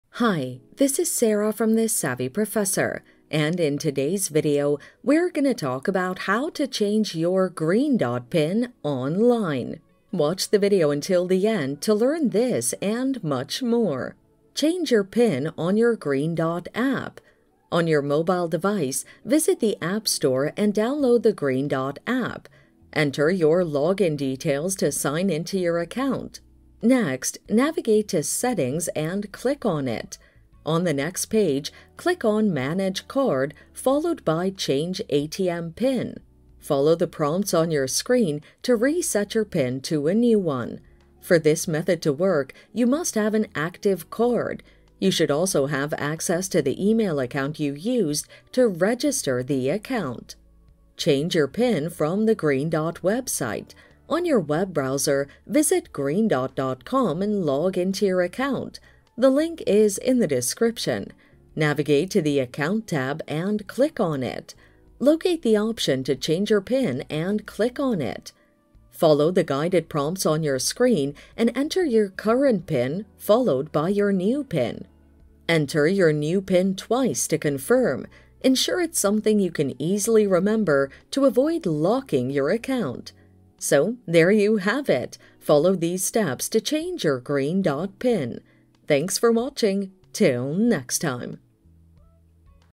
How To Change PIN On Your Green Dot (How Do I Reset My Green Dot PIN Number?)
How To Change PIN On Your Green Dot (How Do I Reset My Green Dot PIN Number?). In this video tutorial I will show how to change PIN on your Green Dot.

Change Pin on Green Dot App
⦁ On your mobile device, visit app store and download the Green Dot app.
⦁ Enter your log in details to sign in to your account.
⦁ Next, navigate to ‘Settings’ and click on it.
⦁ On the next page, click on ‘Manage Card’ followed by ‘Change ATM PIN’.
⦁ Follow the prompts on your screen to reset your PIN to a new one.
For this method to work, you must have an active card. You should also have access to the email account you used to register the account.

Change Pin from Green Dot Website
⦁ On your web browser, visit greedot.com (The link is in the description) and log in to your account.
⦁ Navigate to the 'Account' tab and click on it.
⦁ Locate the option to change your PIN and click on it.
⦁ Follow the guided prompts on your screen and enter your current PIN, followed by your new PIN.
⦁ Enter your new Pin twice to confirm. Ensure it is something you can easily remember to avoid locking your account.